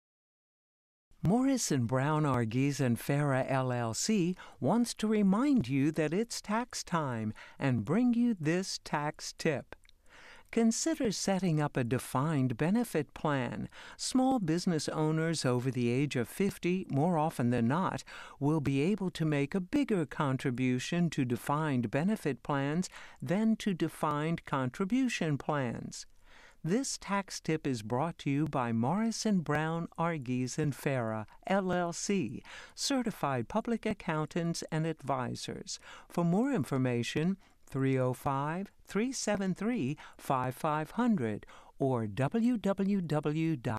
Tax Tip 2012 - Defined Benefit Plan
Consider setting up a defined benefit plan. Small business owners over the age of 50 more often than not will be able to make a bigger contribution to defined benefit plans...